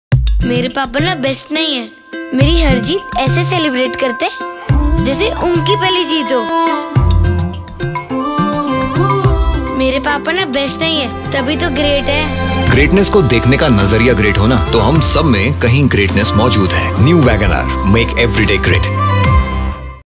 Make Everyday Great | Celebrating Greatness with WagonR
Greatness of the WagonR man lies in the little acts of goodness he does every day. From planning heartfelt surprises to celebrating small victories, he is a great man with a heart of gold.
Just like the all-new WagonR, that is great in every aspect. Experience great style, great comfort and great performance with WagonR VXi and VXi+.